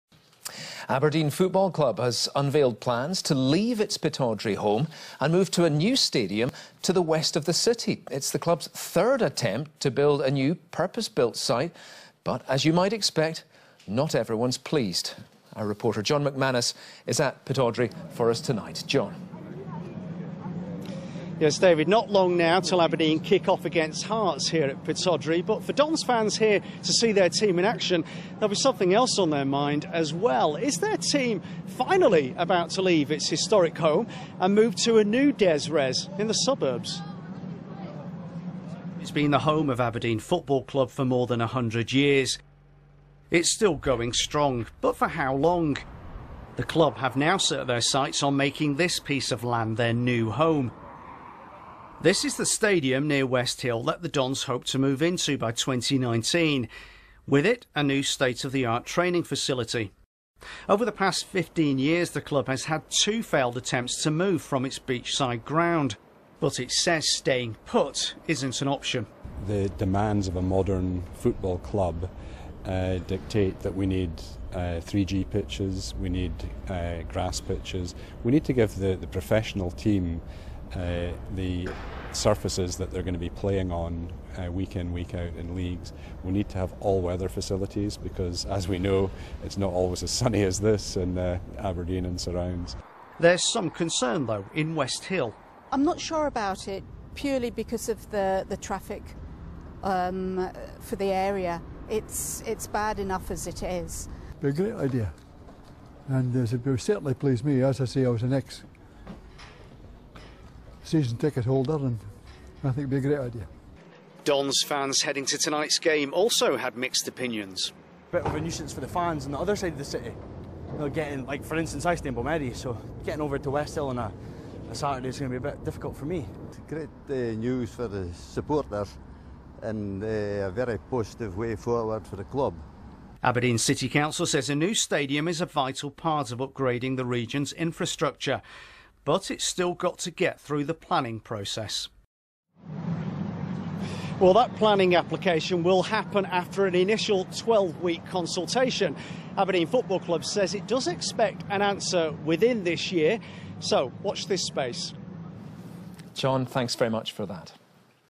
Aberdeen FC say they'll move from Pittodrie.
Will Aberdeen FC move from their historic Pittodrie home? Live and package for BBC News.